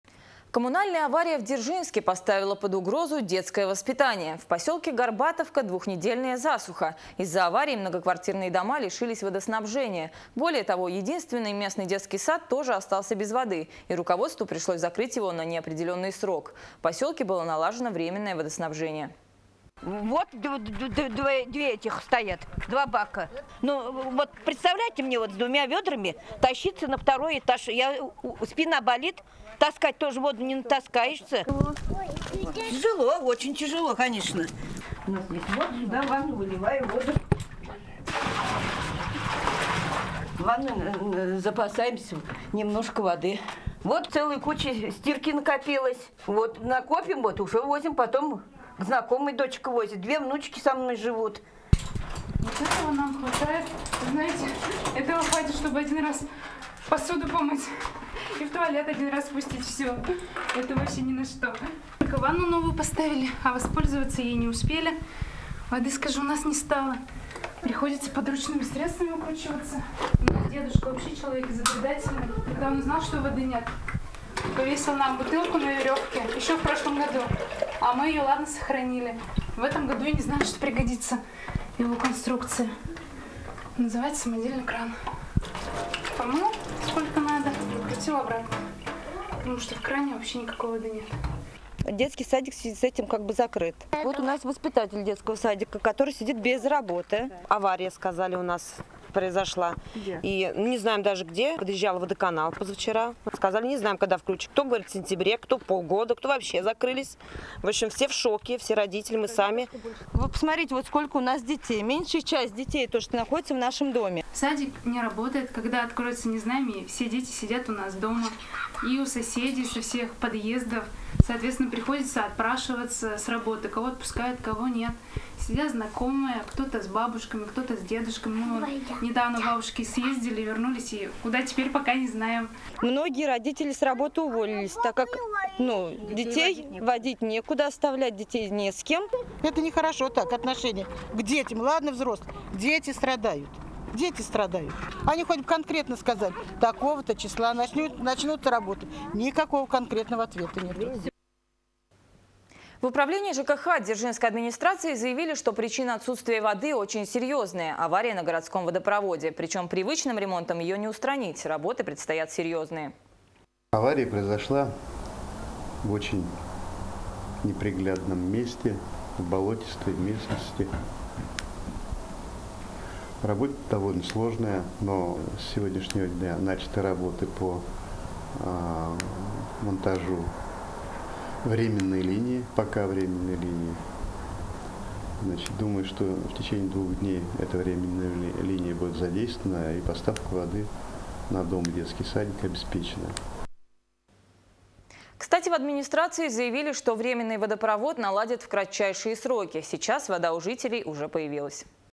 Дети Горбатовки остались без дошкольного образования: из-за коммунальной аварии закрыли детсад
Коммунальная авария в Дзержинске поставила под угрозу детское воспитание -  в поселке Горбатовка двухнедельная засуха, из-за аварии многоквартирные дома лишились водоснабжения, более того, единственный местный детский сад тоже остался без воды, и руководству пришлось закрыть его на неопределенный срок. В поселке было налажено временное водоснабжение